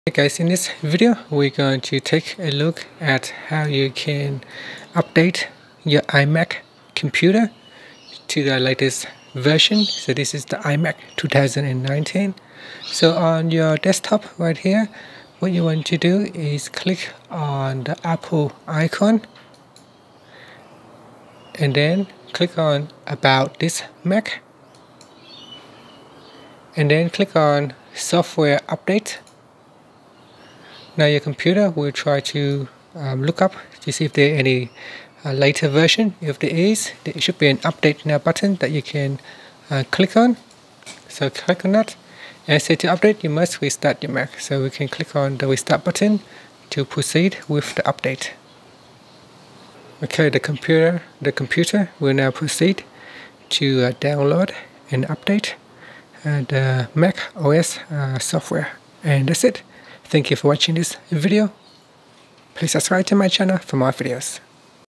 How to Update iMac Software to the Latest macOS Monterey - iMac 2019
Learn how you can update the iMac software to the latest macOS Monterey on the iMac 2019.